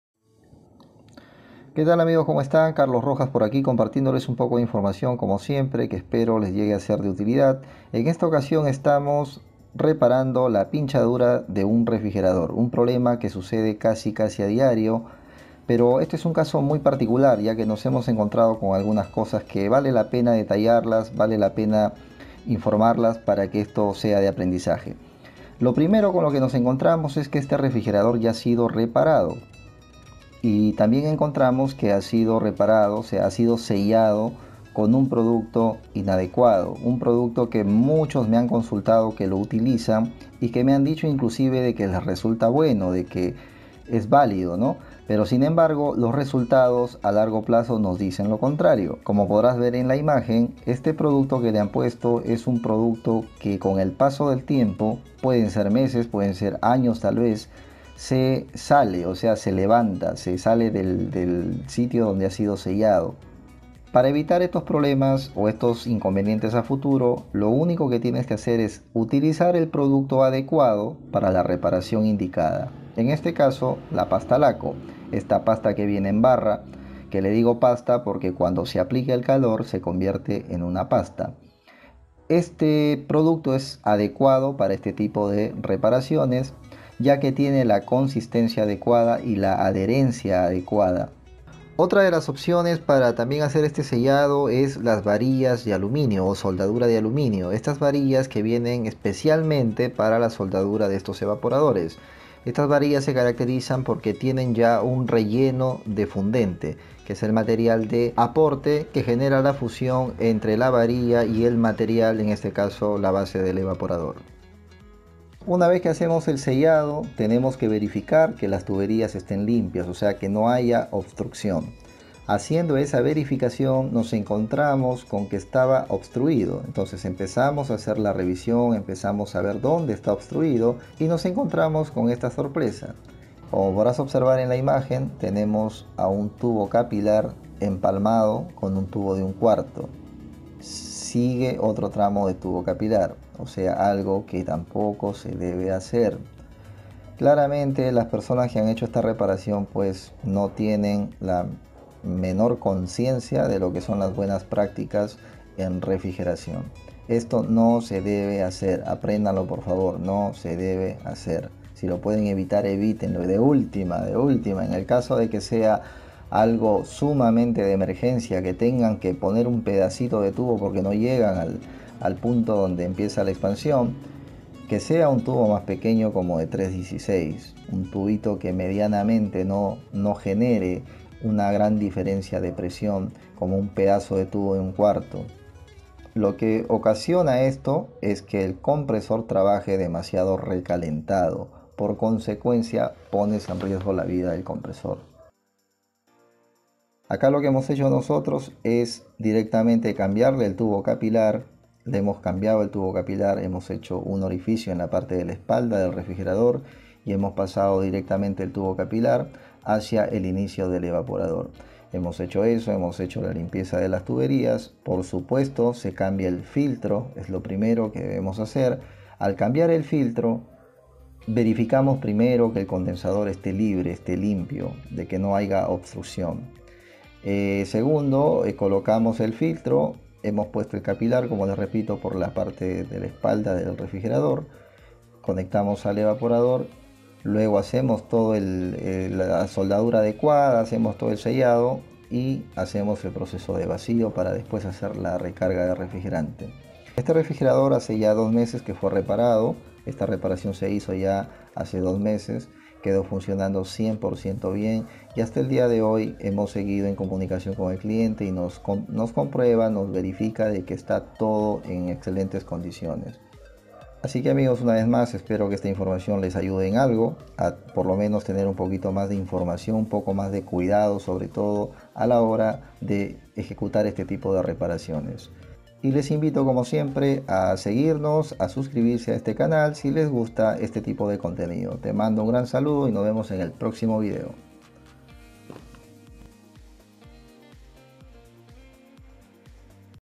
como sellar la pinchadura de los refrigeradores?..neveras ó frigorífico
te compartimos datos prácticos para la reparación de las pinchaduras en los refrigeradores, que materiales usar para sellar y que material NO  se deben usar...